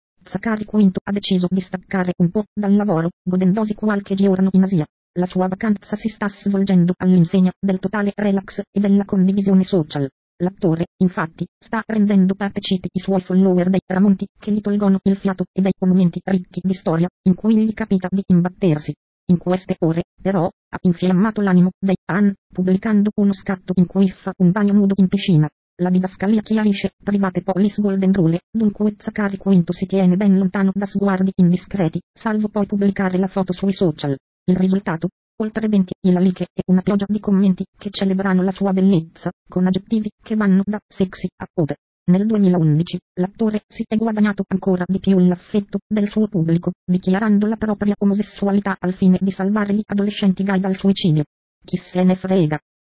Zachary Quinto fa il bagno nudo in piscina, i fan "Sei così sexy"
L'attore ha pubblicato su Instagram, uno scatto che lo ritrae mentre fa il bagno nudo in una piscina privata. I fan hanno molto apprezzato, ricoprendolo di complimenti.